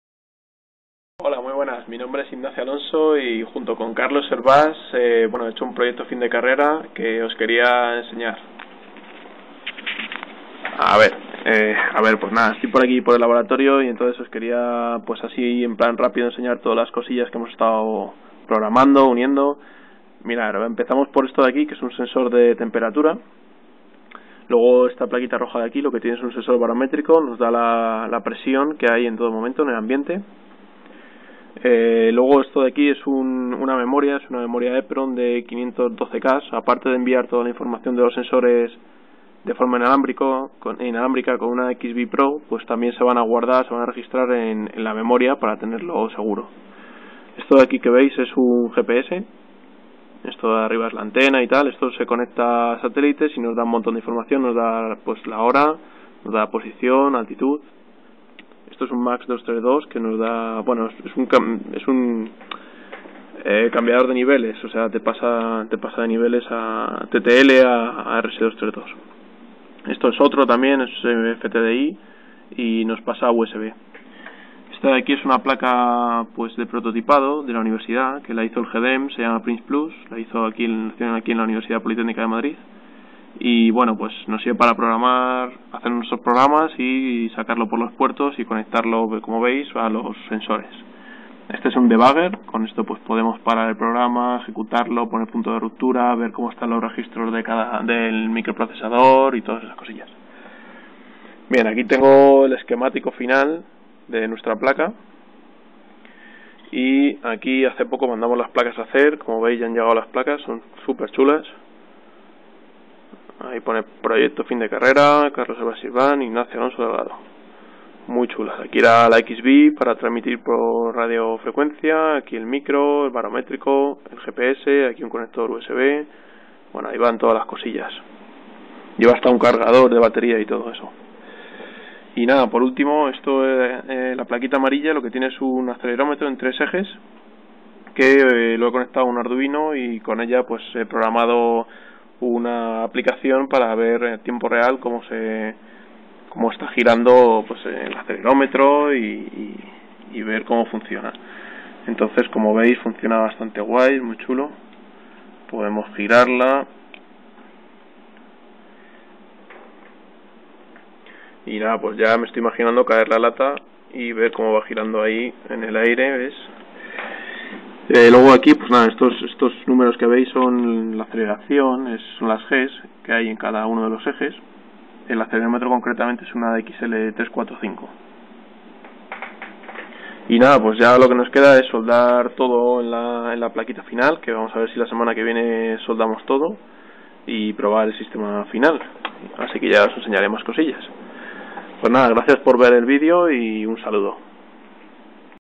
PFC - Ignacio Alonso & Carlos Hervás
"Sistema multisensorial atónomo"

Descripción a grandes pinceladas del Proyecto Fin de Carrera de Ignacio Alonso y Carlos Hervás.

Sensores de presión, temperatura, GPS, acelerómetro en 3 ejes, memoria EEPROM, transmisor de RF XBee PRO, cargador de batería incluido, microProcesador 80C51F320. Autonomía de más de 20h.

EUITT - UPM